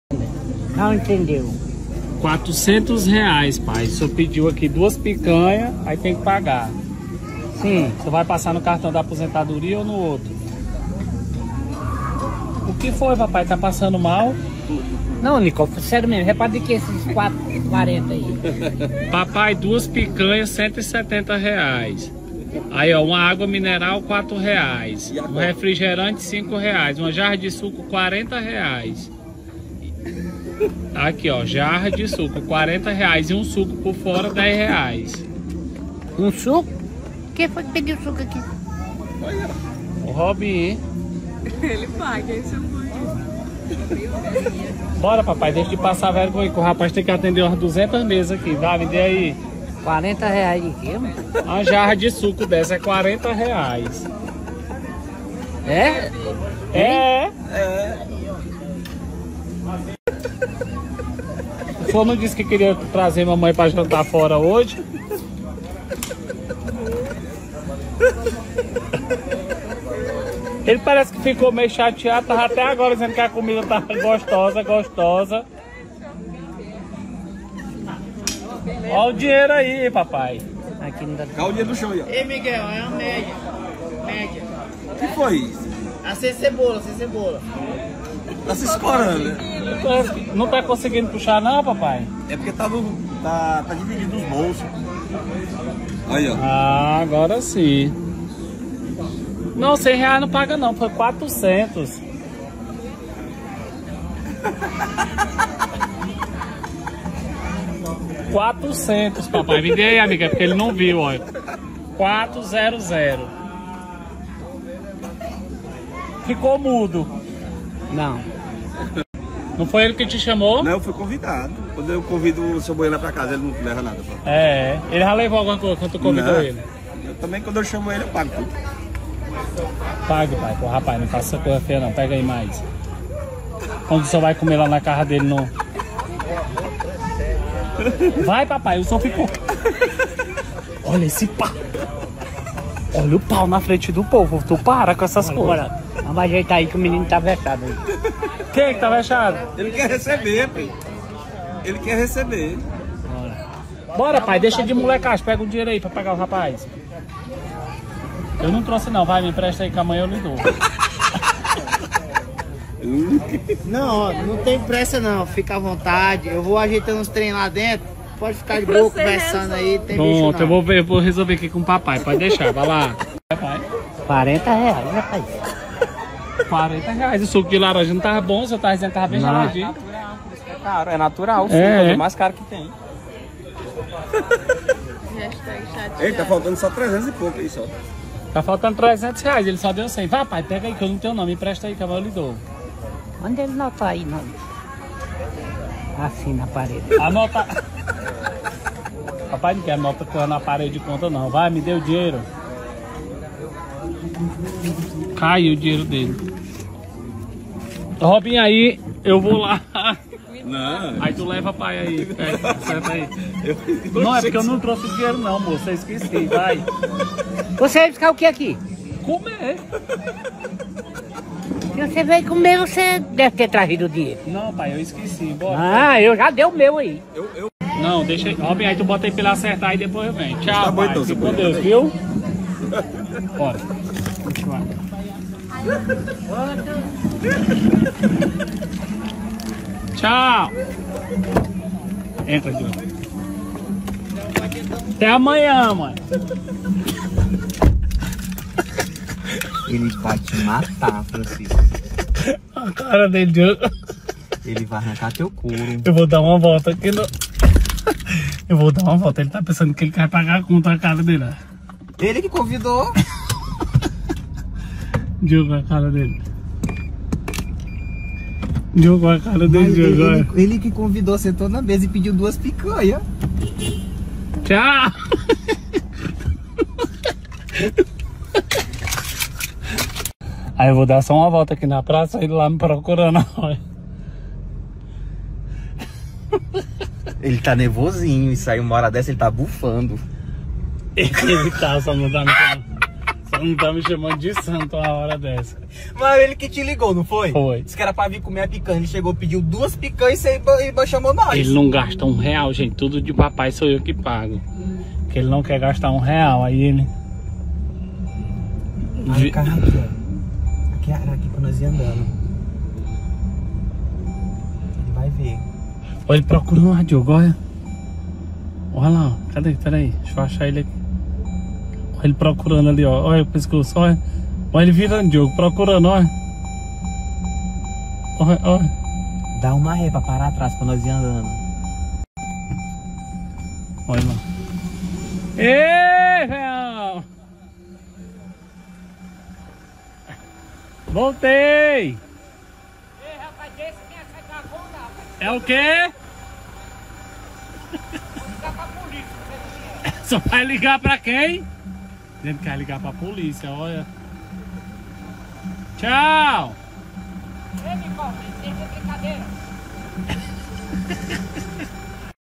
BOINHA CHAMA PARA COMER PICANHA NA HORA DE PAGAR É BABADO!
Boinha nao acreditou quanto deu Nicó saiu de fininho.